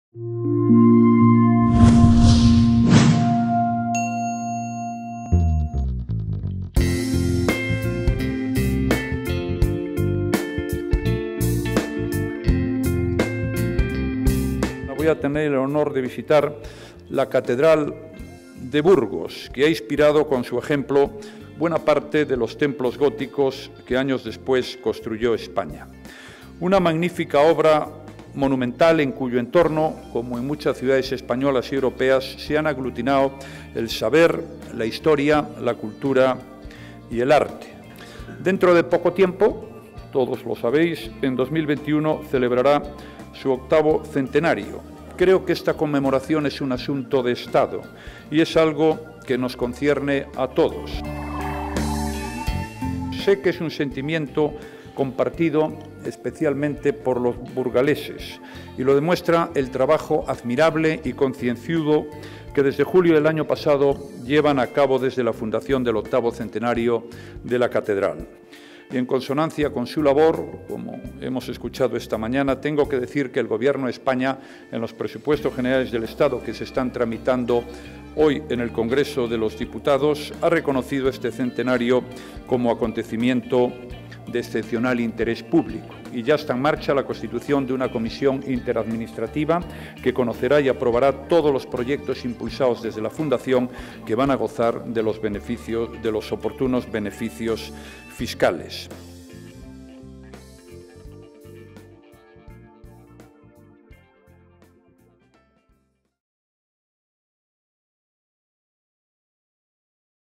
Mariano Rajoy visita la Catedral de Burgos. Universidad de Burgos
Con motivo del próximo centenario de la Catedral de la capital burgalesa, el presidente del Gobierno, Mariano Rajoy ha visitado el templo burgalés.